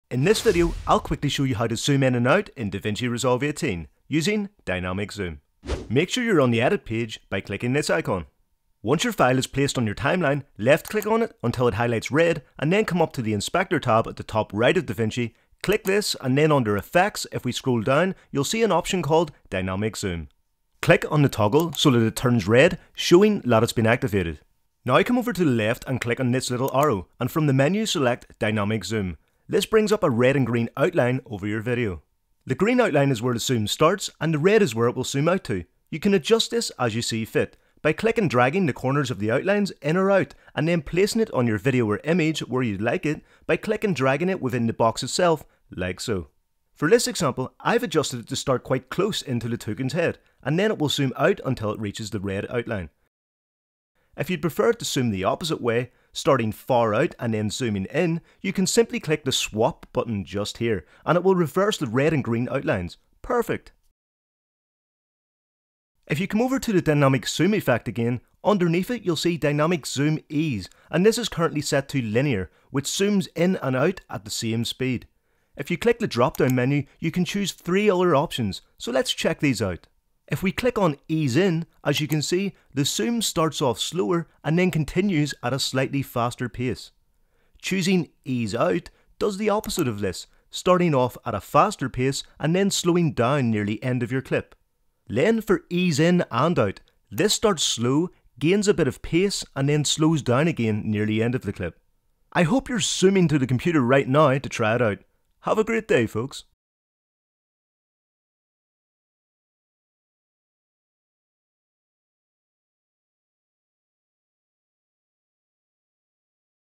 How to ZOOM In DaVinci Resolve 18 (Tutorial)
In this Video, I'll show you how to zoom in DaVinci Resolve 18. This quick and easy tutorial is aimed for beginner's, however everyone at any level can make use of this.

By the end of this tutorial, everyone will know how to zoom in on davinci resolve 18.

Creating this Zoom effect is one of the easier DaVinci Resolve transitions available, though it's very effective at the same time.

Import your own image and follow along with me to create one of the easiest DaVinci Resolve effects.

CHAPTERS
00:00 - Intro
00:06 - Toggling on Dynamic Zoom
00:25 - How to use Dynamic Zoom
01:06 - Adjust Dynamic Zoom "Ease" settings
01:40 - Outro